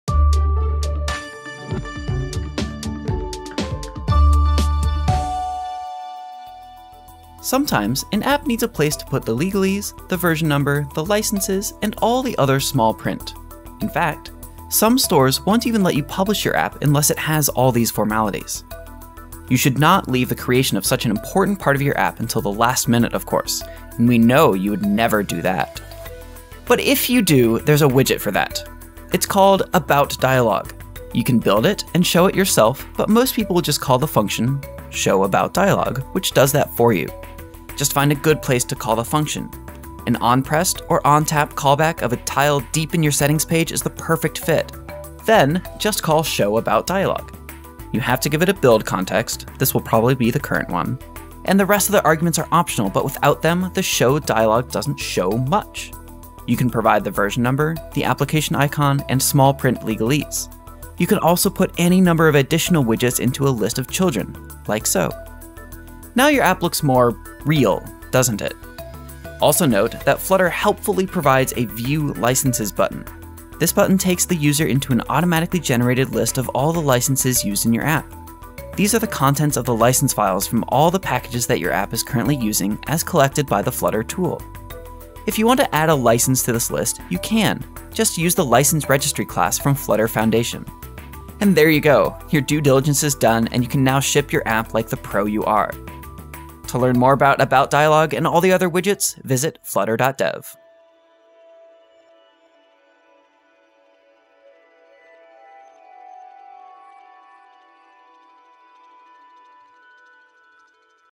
AboutDialog Flutter Widget of the Week
In this flutter tutorial we will use dart to create our first flutter mobile app. You will learn about how flutter work.